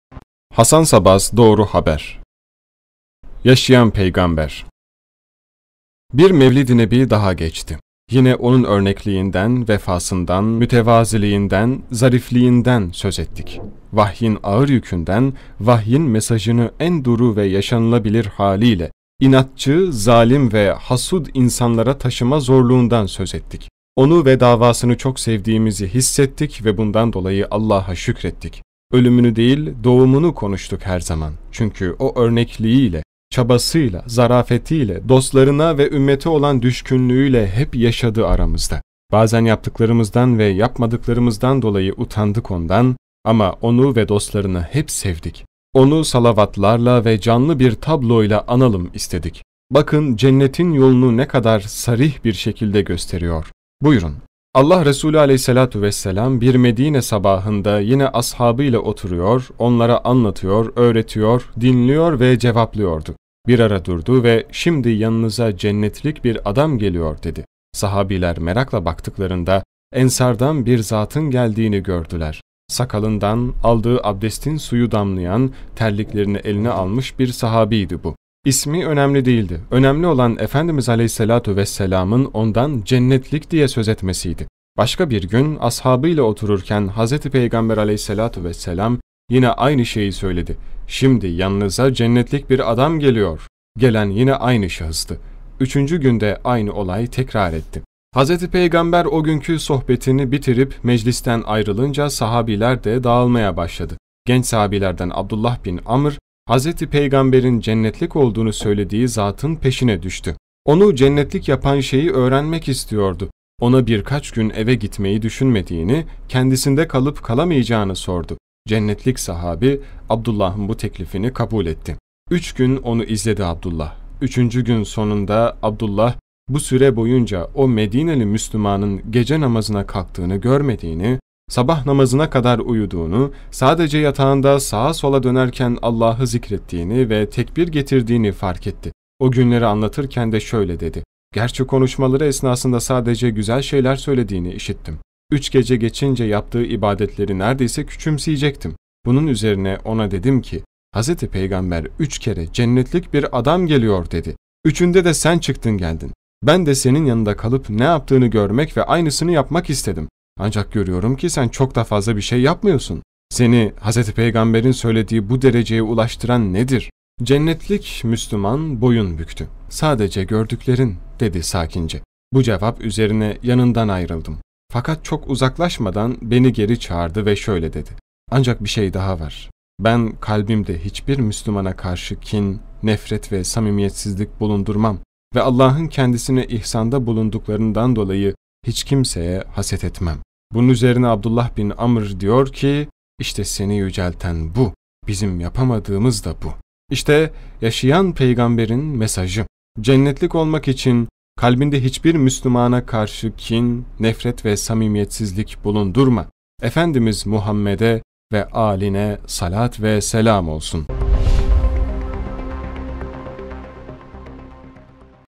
YAŞAYAN PEYGAMBER
HASAN SABAZ / DOĞRUHABER

Bir Mevlid-i Nebi daha geçti.

Yine onun örnekliğinden, vefasından, mütevaziliğinden, zarifliğinden söz ettik.

Vahyin ağır yükünden, vahyin mesajını en duru ve yaşanılabilir haliyle, inatçı, zalim ve hasud insanlara taşıma zorluğundan söz ettik.

O’nu ve davasını çok sevdiğimizi hissettik ve bundan dolayı Allah’a şükrettik.

Ölümünü değil, doğumunu konuştuk her zaman, çünkü O örnekliğiyle, çabasıyla, zarafetiyle, dostlarına ve ümmete olan düşkünlüğüyle hep yaşadı aramızda.

Bazen yaptıklarımızdan ve yapmadıklarımızdan dolayı utandık O’ndan; ama O’nu ve dostlarını hep sevdik.

   OYNATMA LİSTELERİMİZ